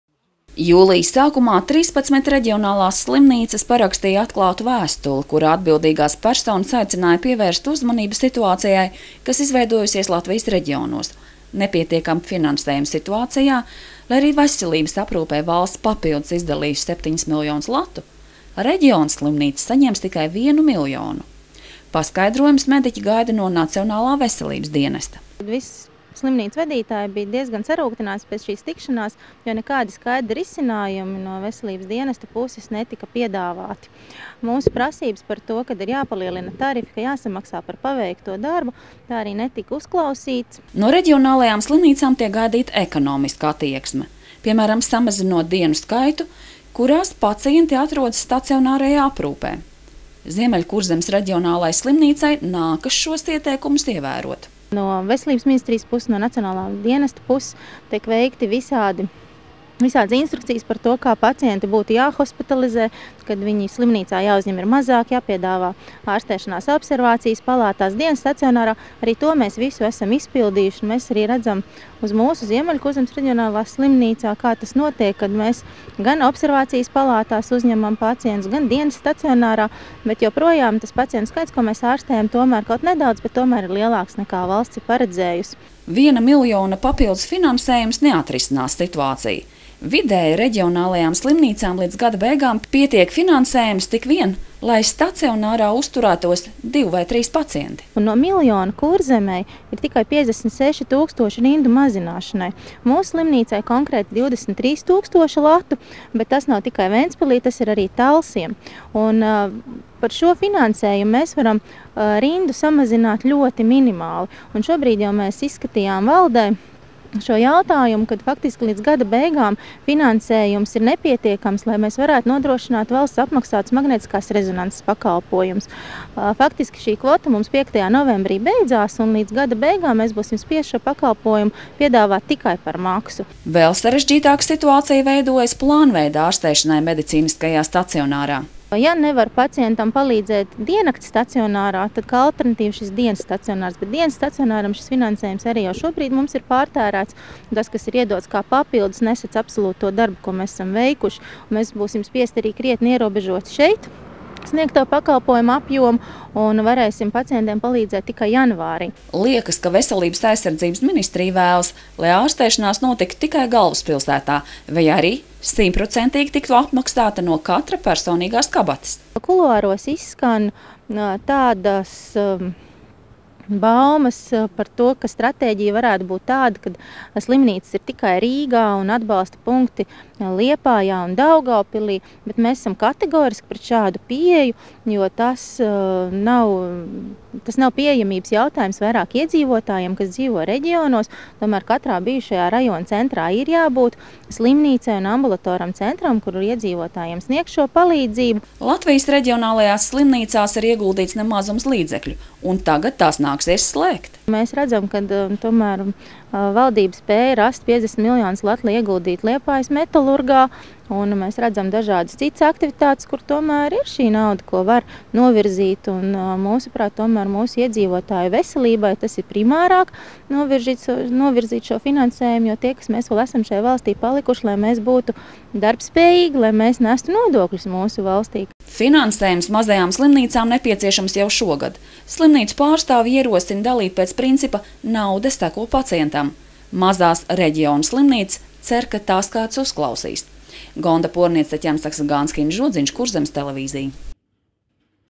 Reģionālās slimnīcas norāda valdībai uz līdzekļu trūkumu
Latvijas reģionālo slimnīcu vadītāju bažas apstiprinājušās. No Nacionālā veselības dienesta saņemtie līgumu piedāvājumi apliecina, ka no papildus piešķirtajiem 7 miljoniem latu veselības nozarei šogad reģionu slimnīcām atvēlētas tikai kapeikas, teikts pirmdien izplatītajā Latvijas Slimnīcu biedrības paziņojumā.